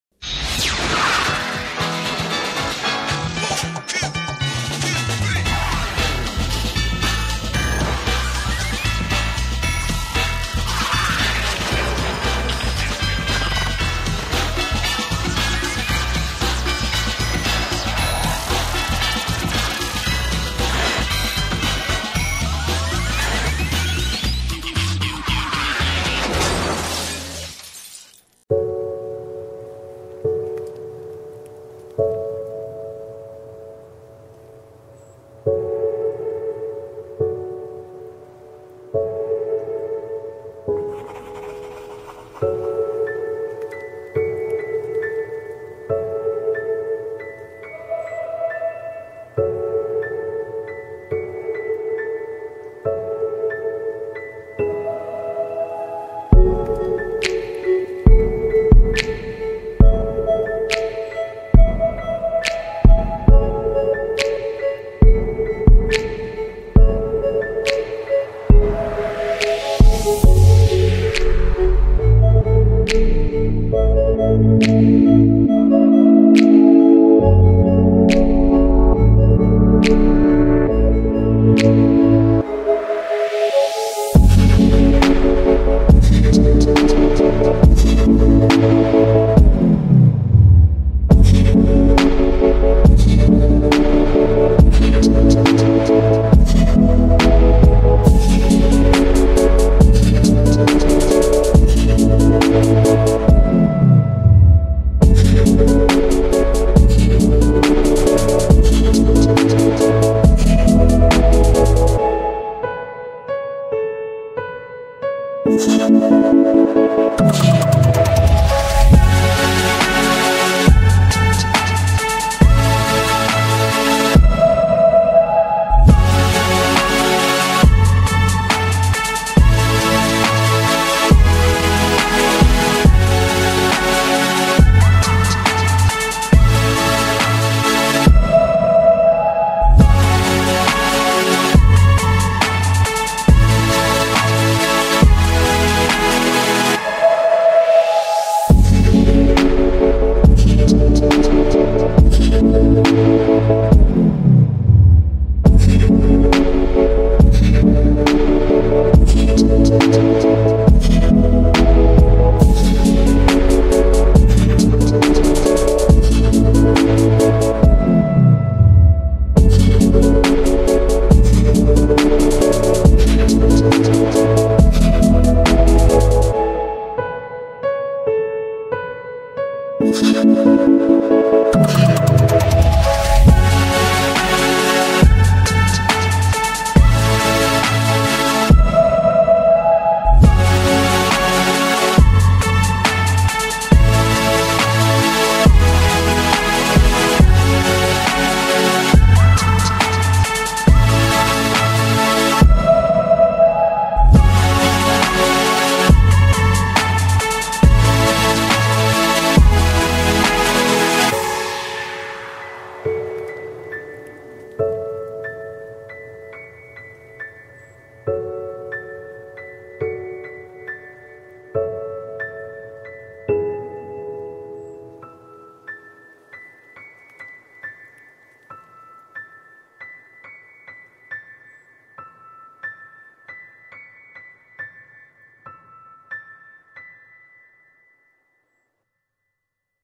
Kerbin Express... Our Crew Is Replaceable. Your Package Isn't.
Jeb and Bill set forth to deliver a package to Omicron Persei 8.